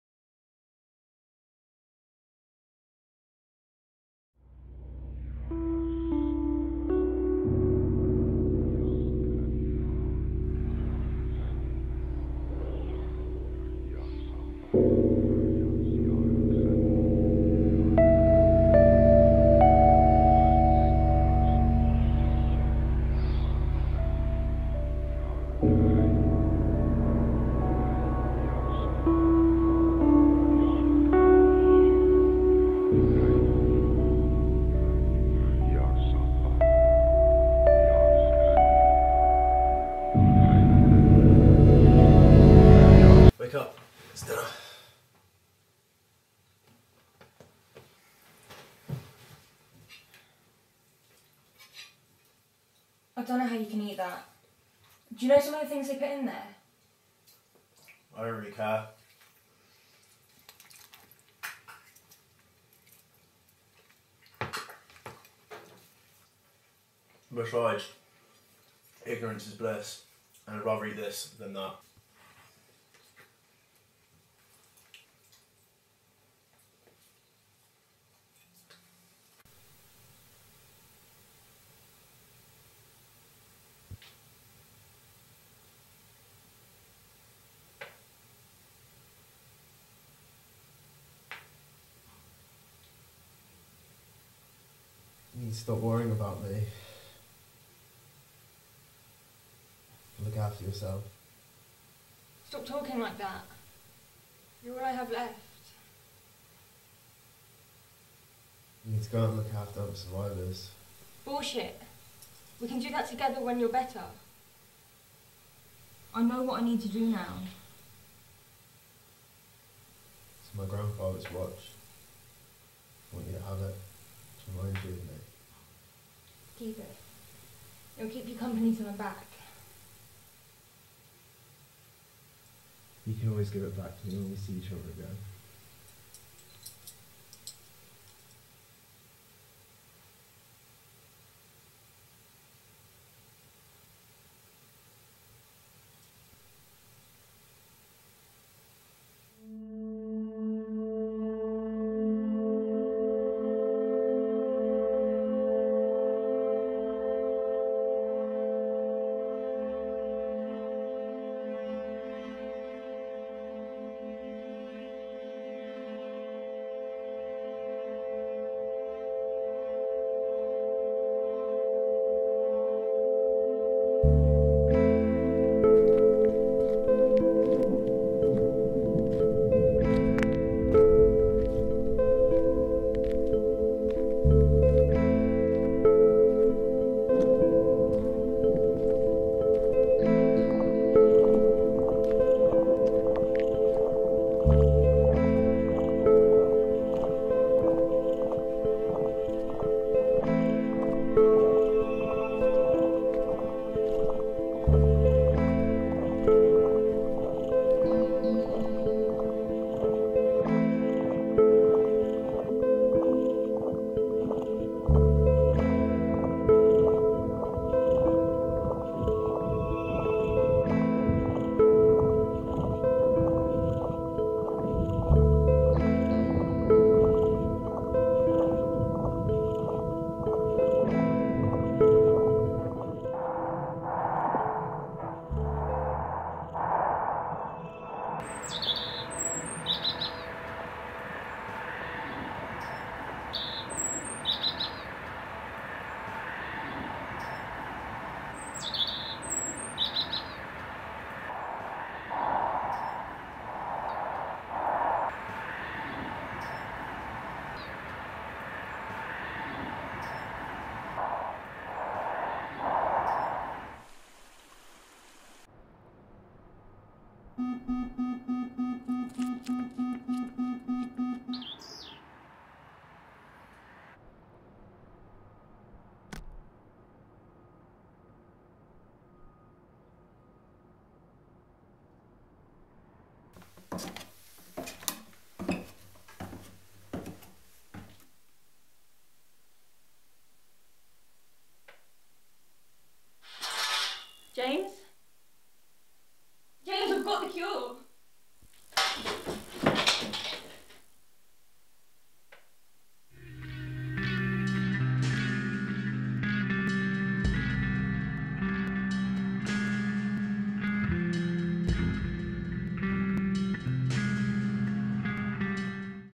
Farmageddon
A 5-min short film created me and my university group

Set in the near future, food contaminated with horse meat has infected humanity, causing death and destruction

-Credits-

-Director-
Andrew Bland
Nick Brouard
Christian Mendoza
Scott Deacon

-Producer-
Andrew Bland
Nick Brouard
Christian Mendoza
Scott Deacon

-Alice-
Jodie Pattle

-James-
Nick Brouard

-Corpse-
Christian Mendoza

-Horses-
Nick Brouard
Christian Mendoza
Scott Deacon

-Cameramen-
Andrew Bland
Nick Brouard
Christian Mendoza
Scott Deacon

-Sound-
Andrew Bland
Nick Brouard
Christian Mendoza
Scott Deacon

-Editing-
Andrew Bland
Nick Brouard
Christian Mendoza
Scott Deacon

-Props-
Scott Deacon

-Makeup-
Christian Mendoza

-Photography-
Christian Mendoza

-Scriptwriter-
Andrew Bland

-Storyboarding-
Scott Deacon